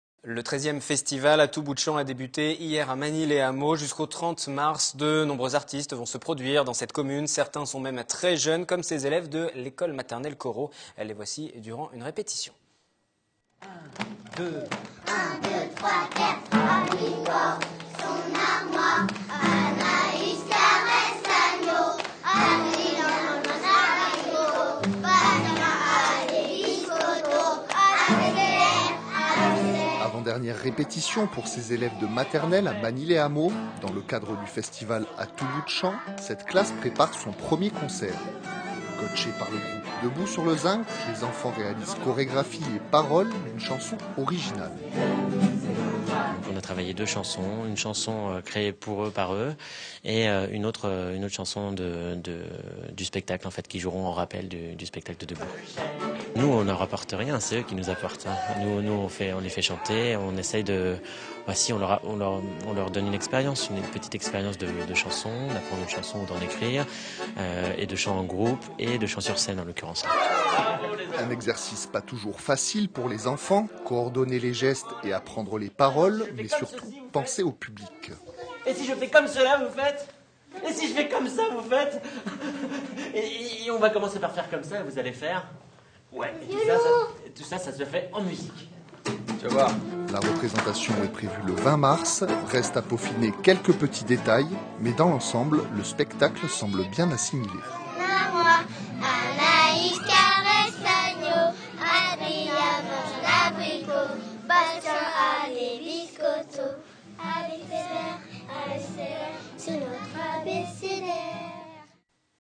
Ateliers Bodysong enfants avec DEBOUT SUR LE ZINC (Magny-les-Hameaux)
Atelier musical 'Bodysong', pour pratiquer la musique en groupe, sans autre instrument que sa voix, son corps, et leurs incroyables possibilités sonores !